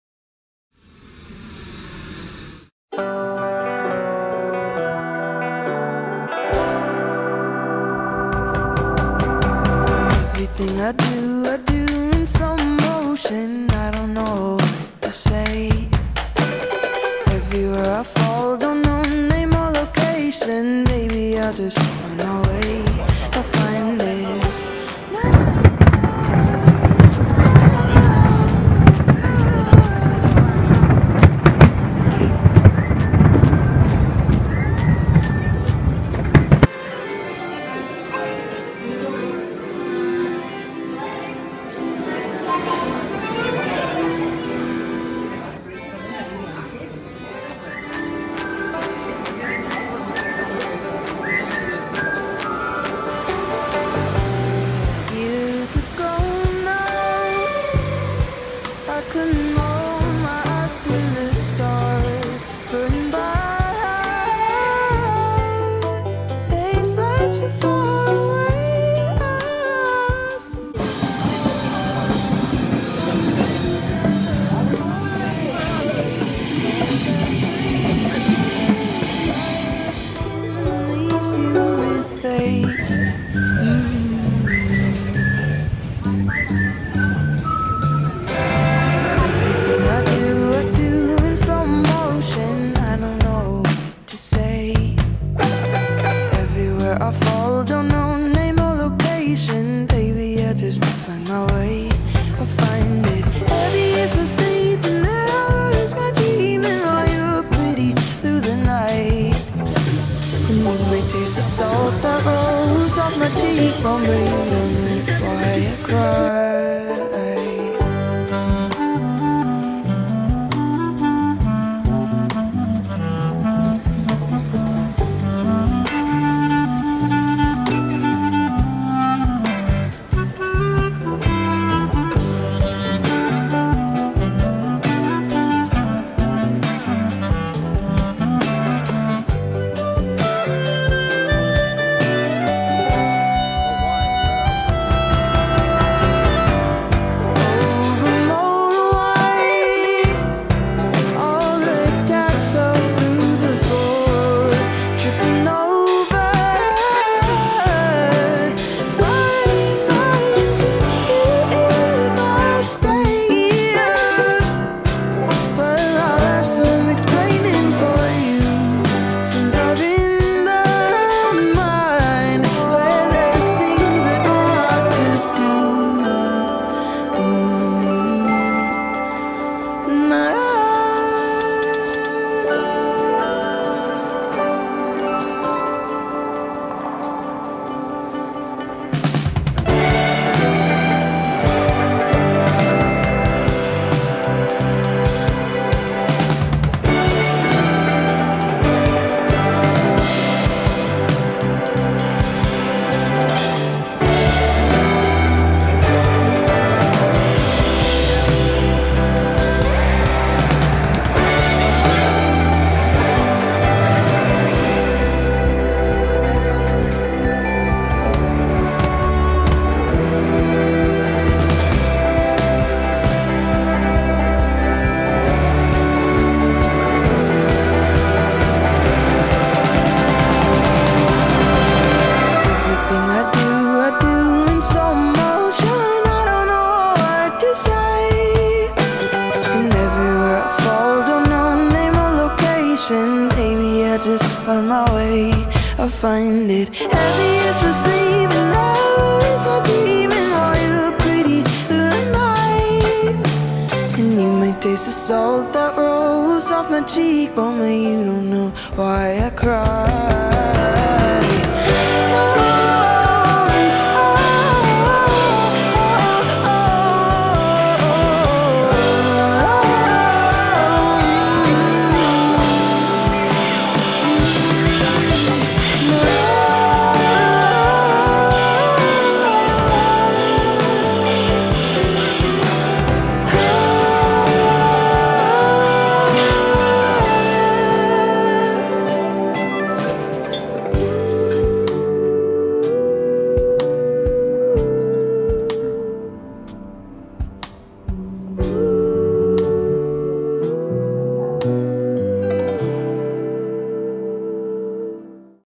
A Summer in New York
I spent the past 5 weeks living in New York City. I had an incredible time exploring the city. This video is a compilation of snippets documenting my time in New York.